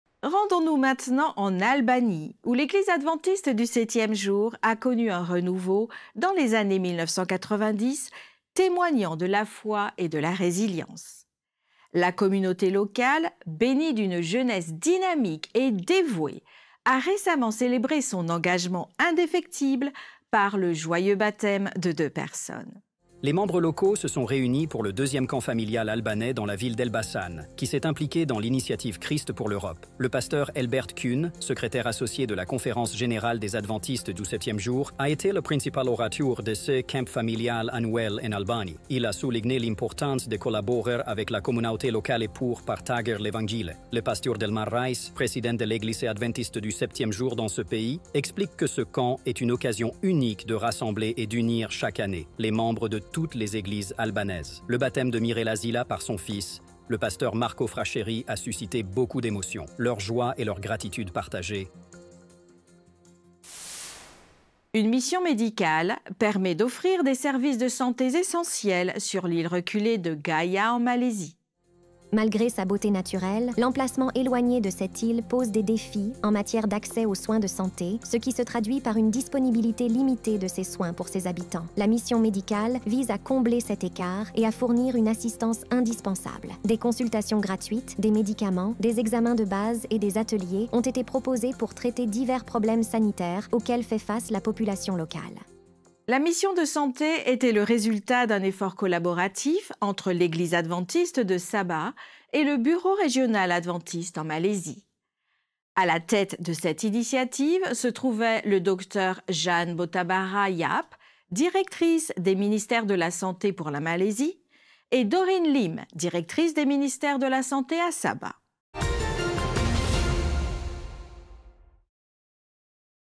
Le deuxième camp familial albanais à Elbasan conduit à deux baptêmes
Des adventistes assistent à des baptêmes au camp familial albanais d'Elbasan.

Ce week-end a également marqué le lancement de la première série de vidéos d'évangélisation en albanais, intitulée "Les enseignements de Jésus".

ANN est la chaîne d'information officielle de l'Église adventiste du septième jour.